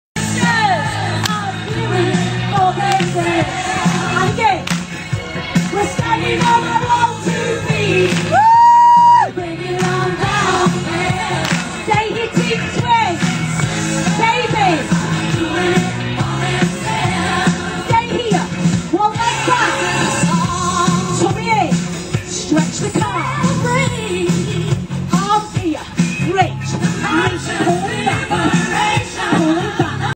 Pink Aerobics - Cherry Baker
Fantastic start to the day with Cherry B at Hyde Park Pink Aerobics event Oct 2011, Wonderful day